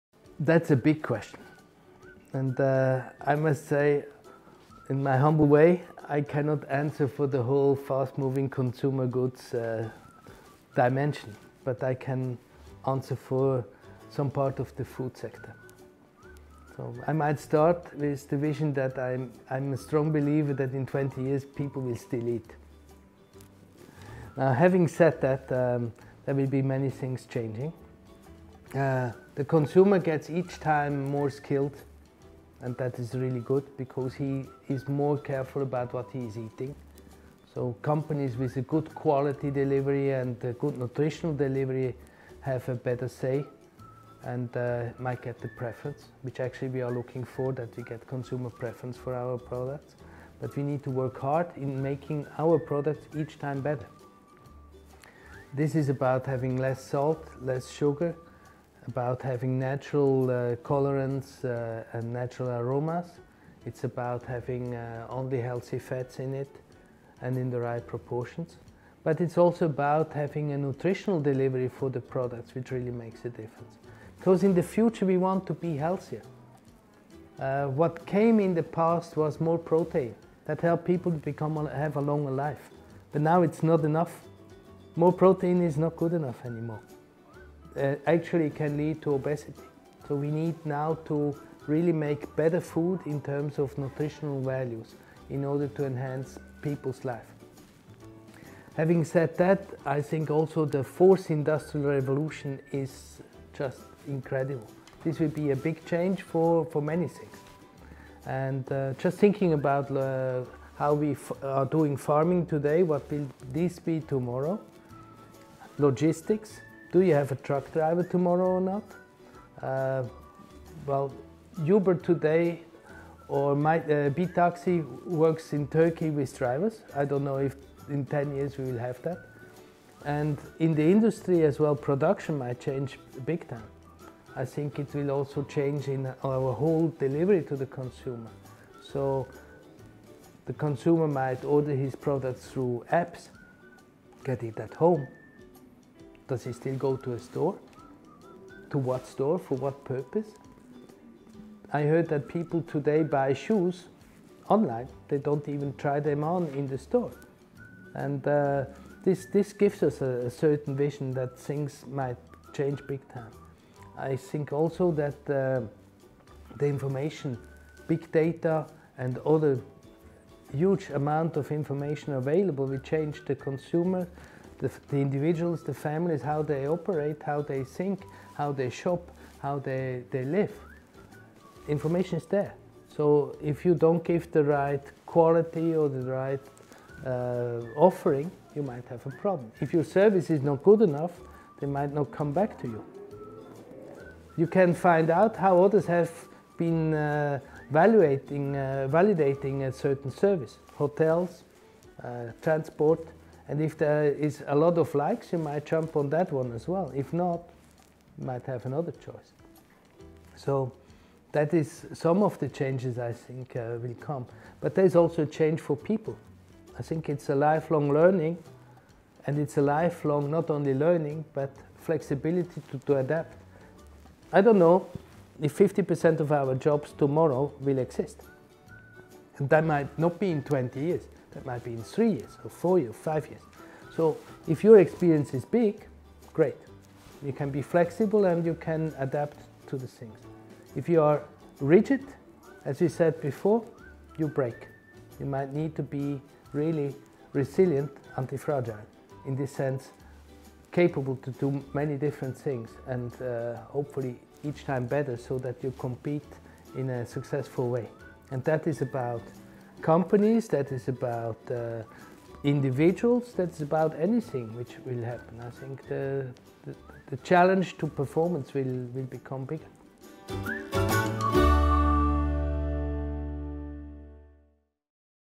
What are your insights on FMCG sector? Where will the sector be in 10-20 years?
Nestlé Genel Müdür / Market Head - Nestlé Turkey Felix Allemann tells us!

What is it like to live and work in Istanbul, Turkey as an expatriate?